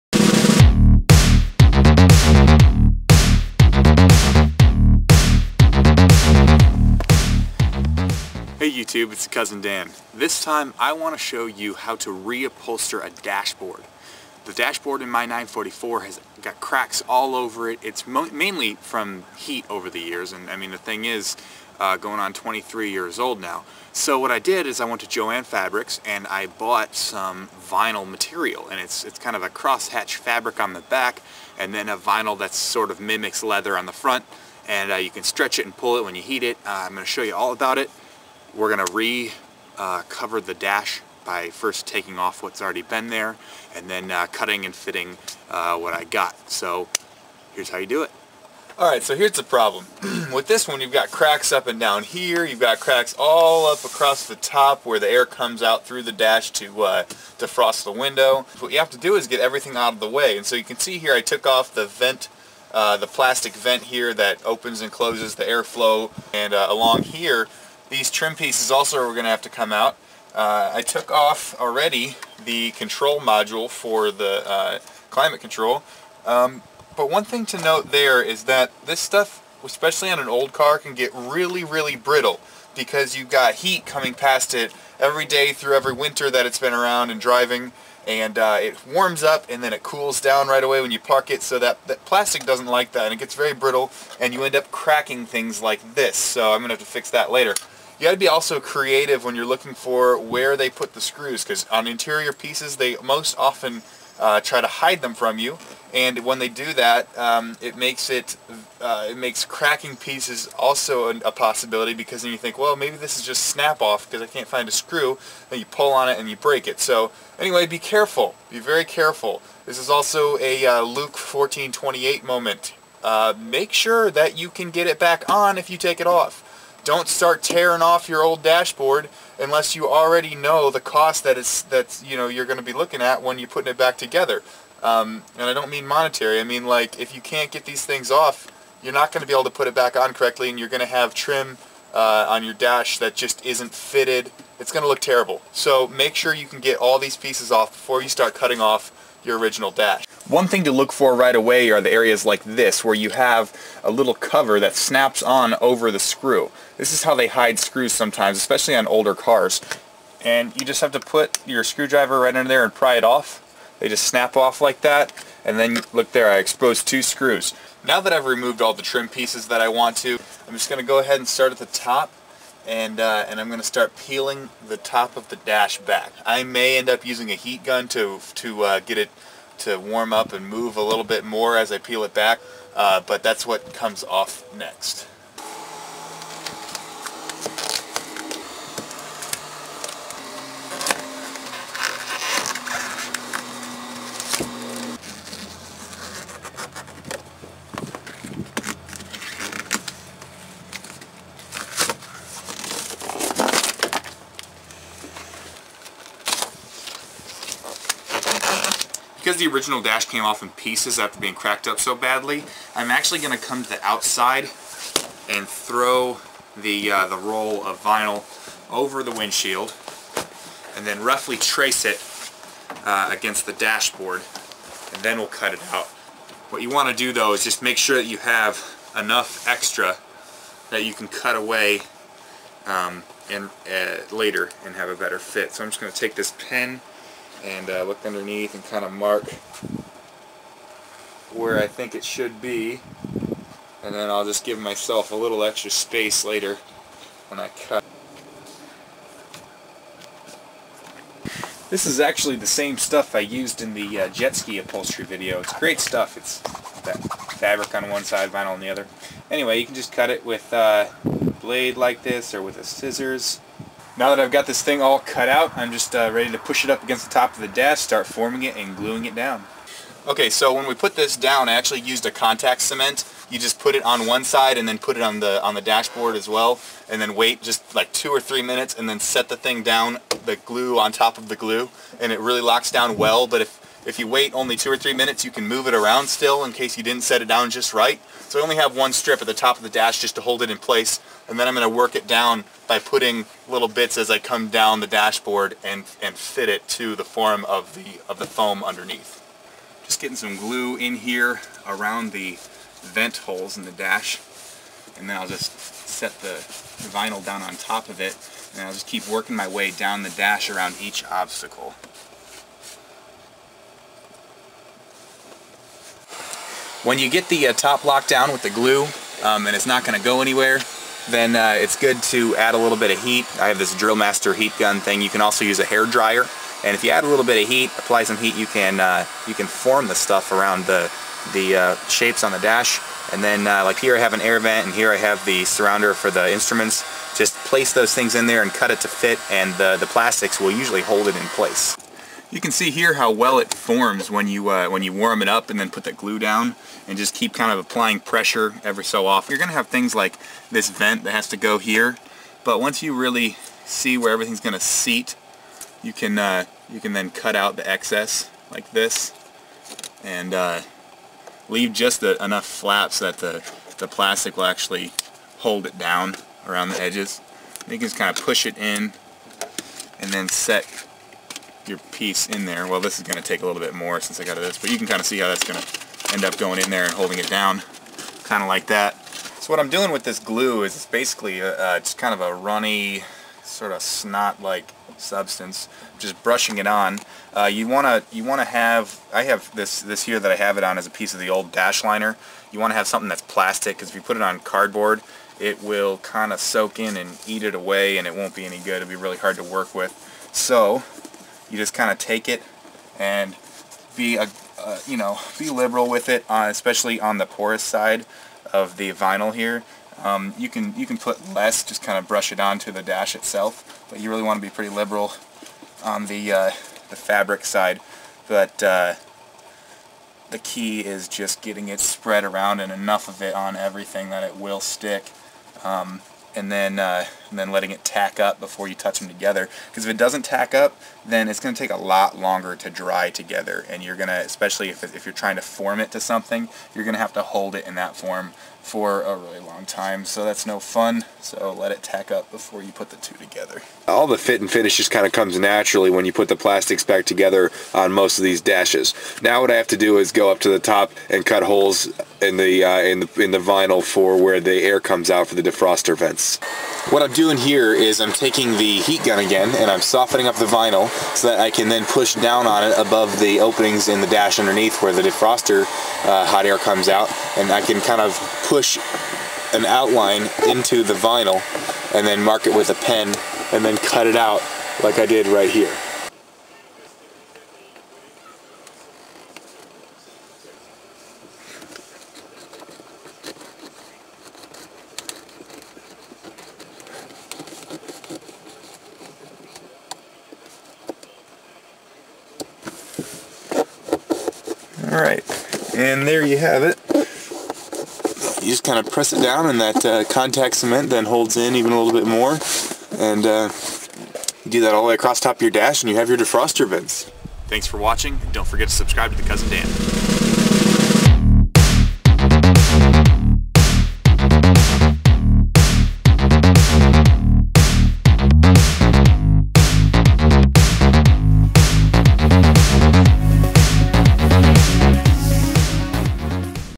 How To Recover A Dashboard - Vinyl, Leather
TheCousinDan shows you how to recover a dashboard with new vinyl.  Restore old cars and trucks with this handy skill!